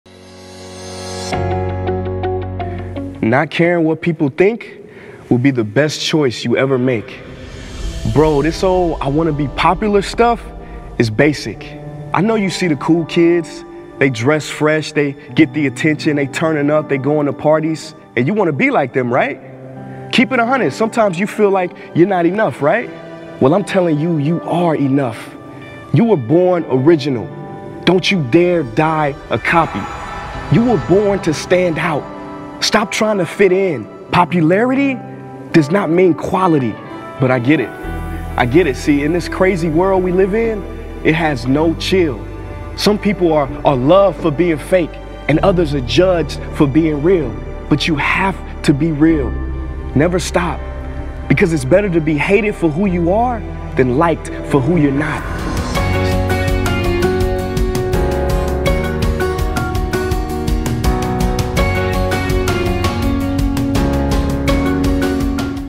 Don't be liked for who you're not
Watch this one minute motivational video courtesy Prince Ea. It's better to be hated for who you are than be liked for who you're not. Be True. Be yourself.